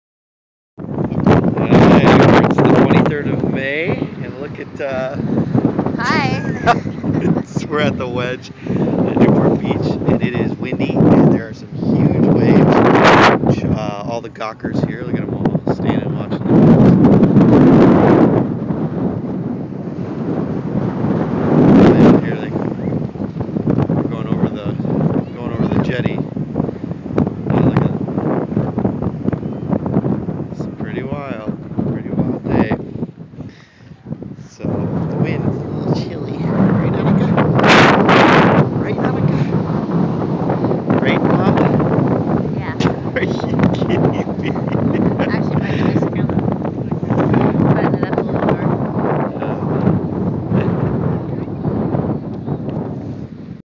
Anna-Lisa Bundled Up at the Wedge in Newport Beach, May 23, 2010
There were hundreds of gawkers like us for the 10-15 foot swells breaking on the jetty.